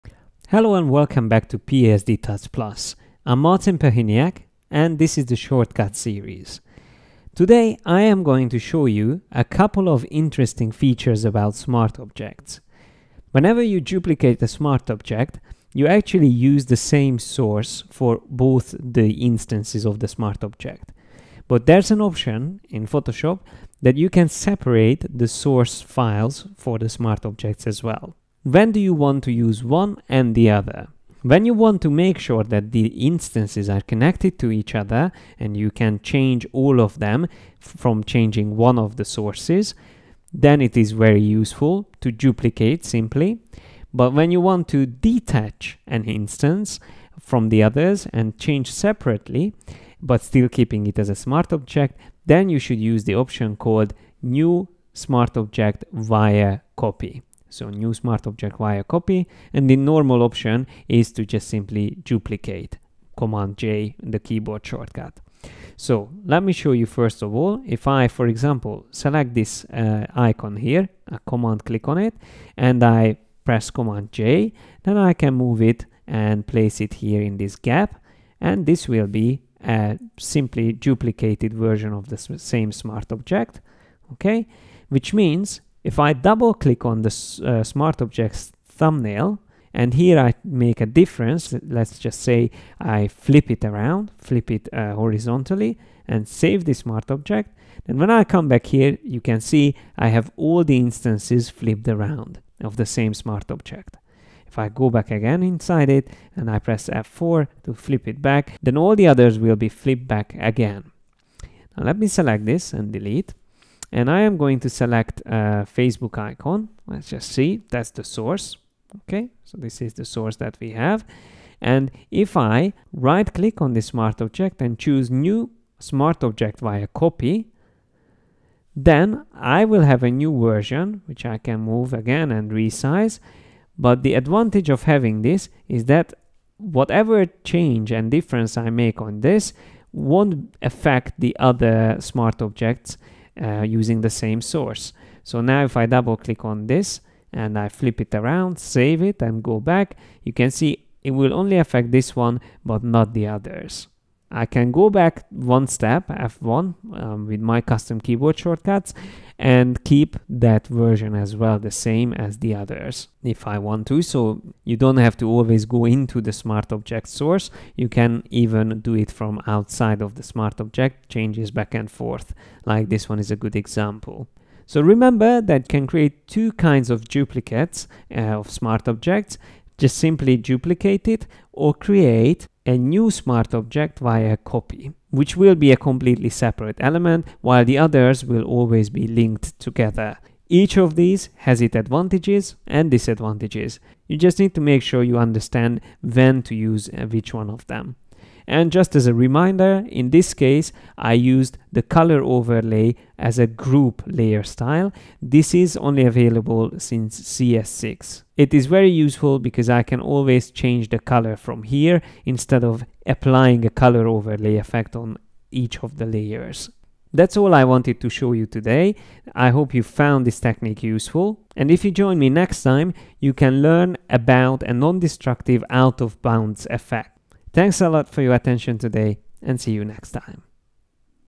Understanding Smart Objects Via Copy
If you would like to work more efficiently in Photoshop, then it is important to know how to make the most of smart objects. This means understanding the difference between simply duplicating a smart object and creating new smart objects via copy. In this tutorial, we will explain the difference between both of these features. Let's get started!